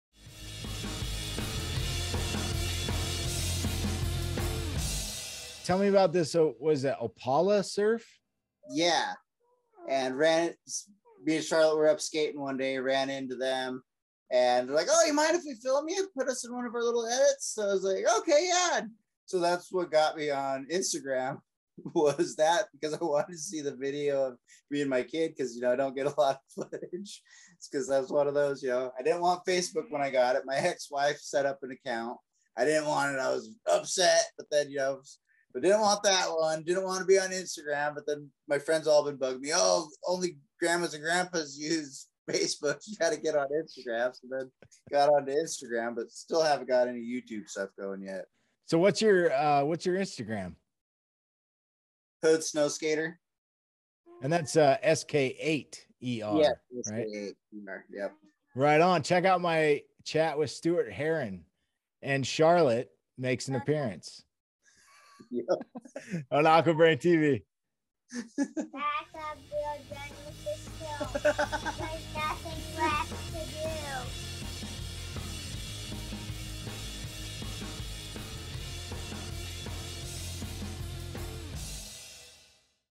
Social Media : Stuart Herrin
Stuart Herrin and Dru Chat about Social media.